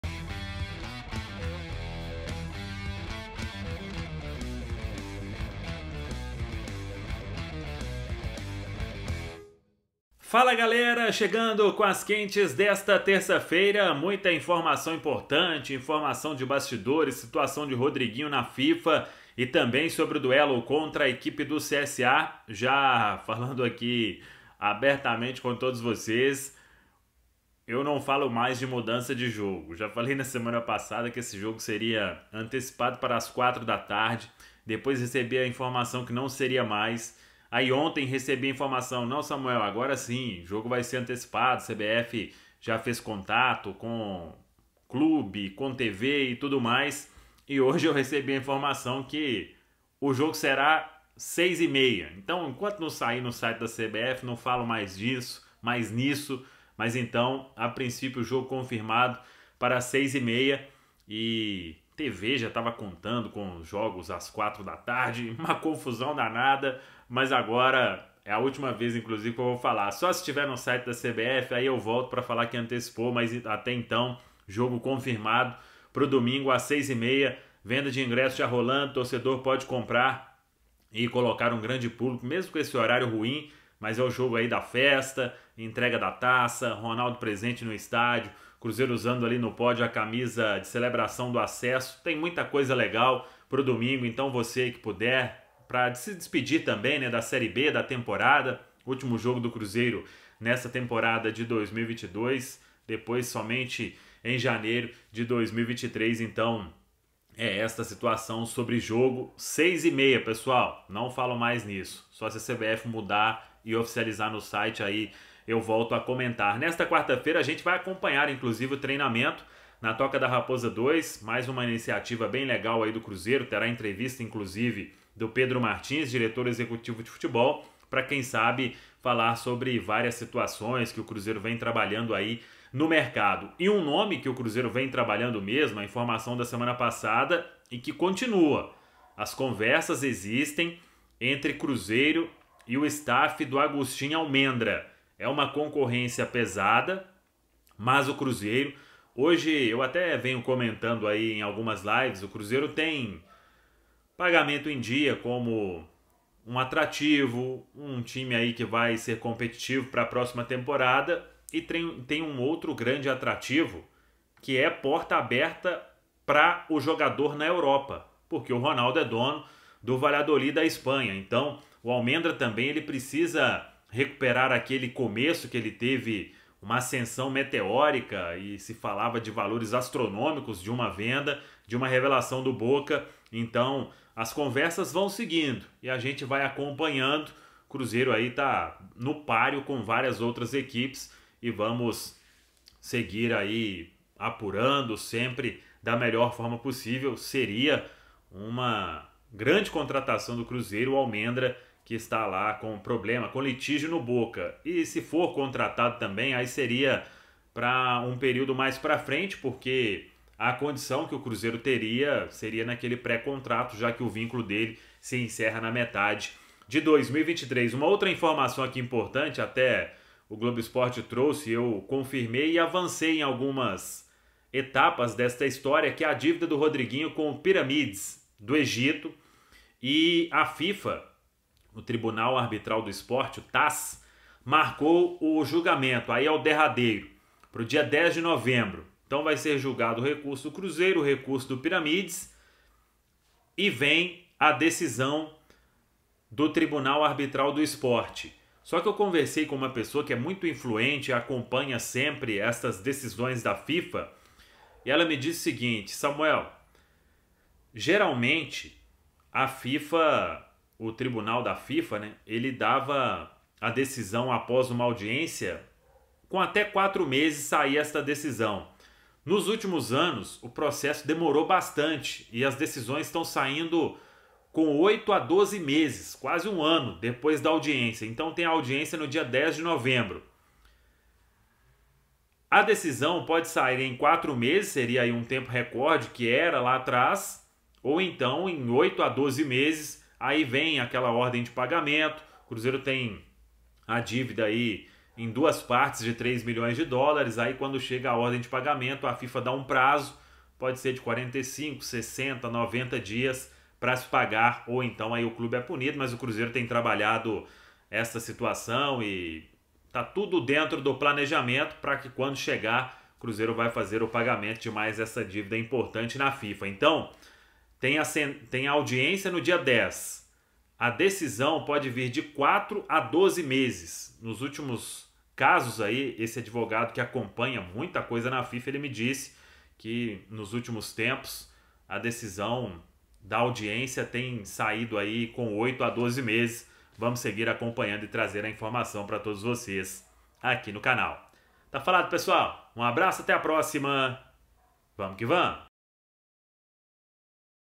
CRUZEIRO SEGUE CONVERSA POR ALMENDRA; QUANDO DEVE SAIR A ORDEM DE PAGAMENTO NA FIFA
Cruzeiro segue conversando em busca da contratação de Almendra, do Boca Juniors. Quando deve sair a ordem de pagamento sobre Rodriguinho na FIFA.

OFERECIMENTO

SUPERMERCADOS BH

KTO

AGUIAR FLEX MÓVEIS

OFFICIAL STORE BETIM